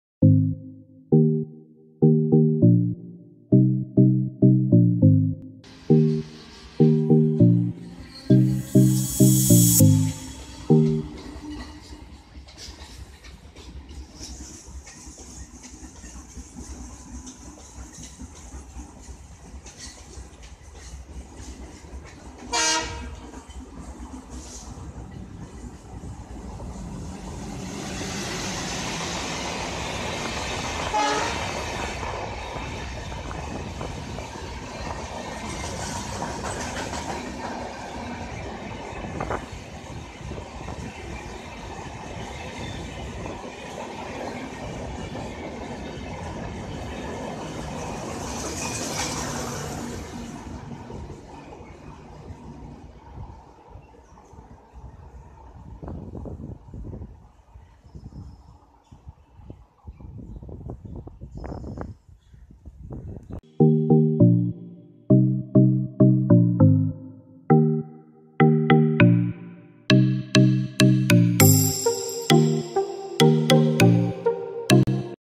01063 Mumbai CST Chennai AC SPL Skip Shivajinagar
01063 Chhatrapati Shivaji Terminus Mumbai- Chennai Egmore  Air-conditioned Special hauled by KALYAN WDM-3D #11455  skipping Shivajinagar  at MPS.